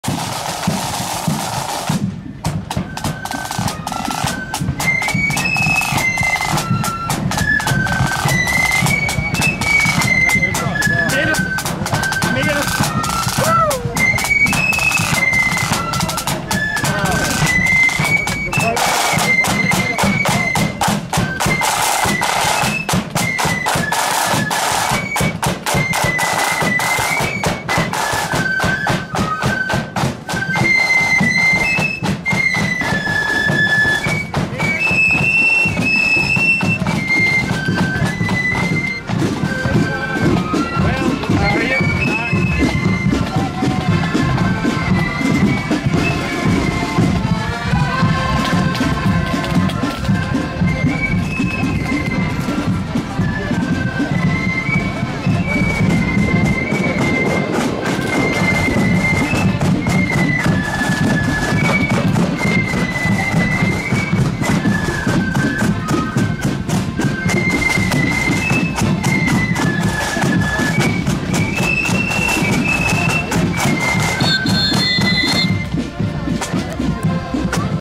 Kilcluney Volunteers (No.12) @ Co. Down Black Saturday Parade ~ Dromore ~ 26/08/23 (4K)
Kilcluney Volunteers Flute Band from Markethill parading through Dromore at the annual Co. Down Black Saturday parade which is always held on the last Saturday in August (hence one of the other names for it "The Last Saturday Parade") which this year fell on Saturday 26/08/2023.